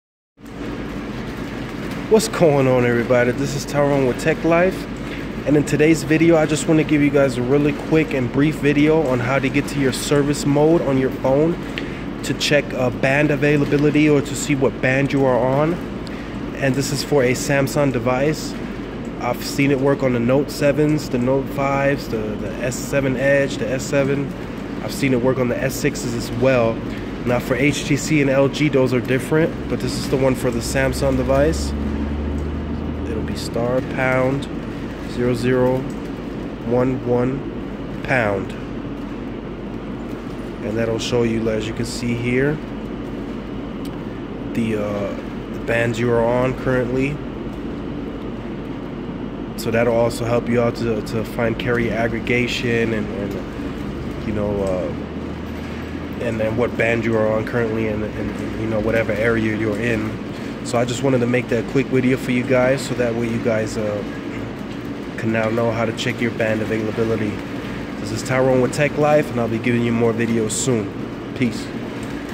How to check what band your phone is connected to (Samsung Galaxy s7) works for (all carriers)
In Today's video im going to show you how to check what band or bands your phone is connected to. This will help you to find out if carrier aggregation is avaliable in you area. This is for samsung's devices only